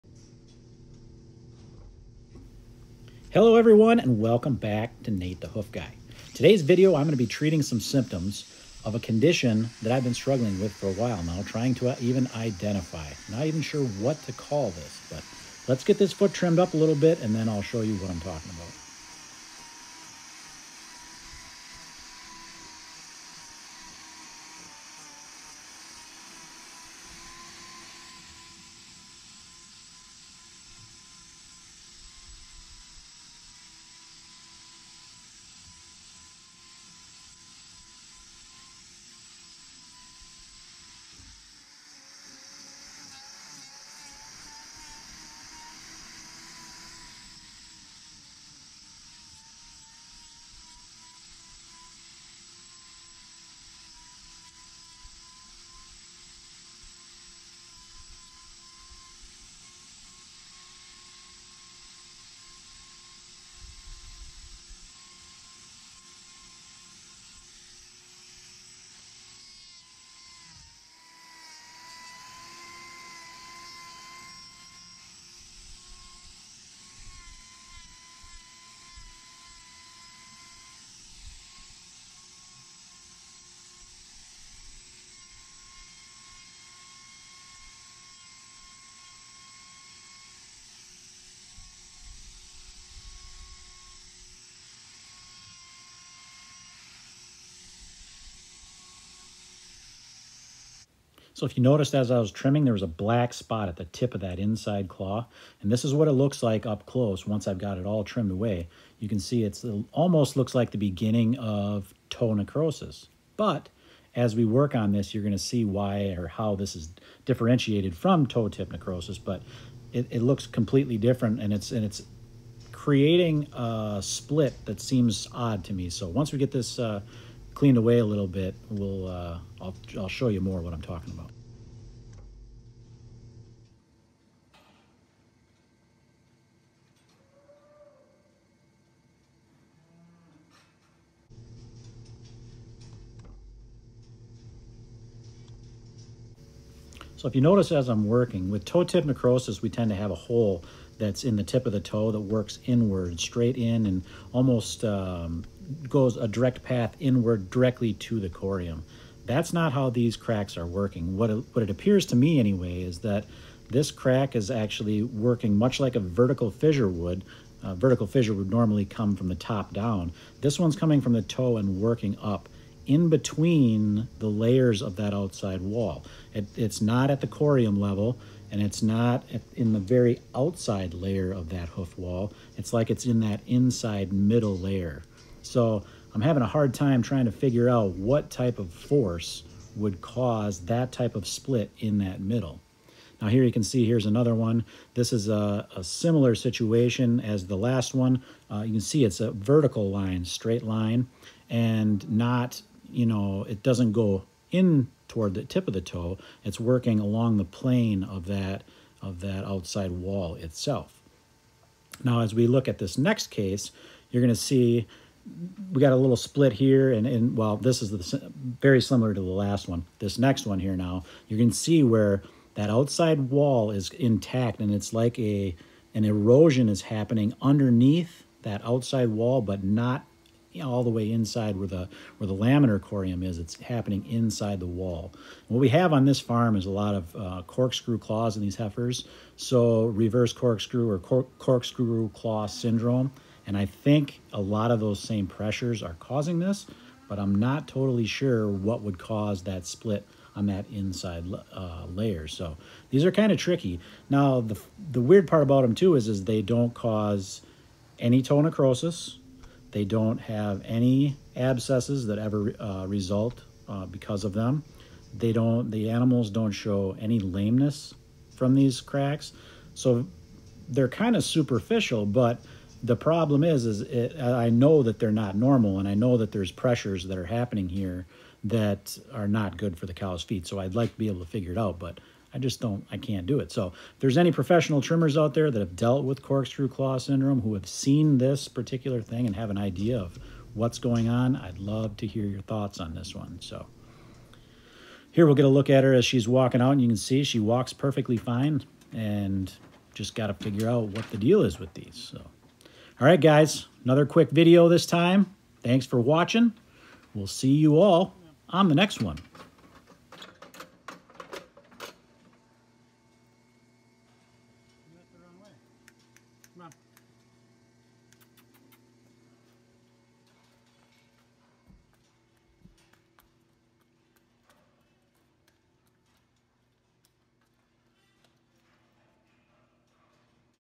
I Don’t Even Know What to Call This
Today’s video we treat symptoms associated with corkscrew claw syndrome in heifers.

Products I use:
Salicylic Acid
Copper Sulphate
Head Gear
Face Shield
Hearing Protection
Bovibond Blocks
Go Pro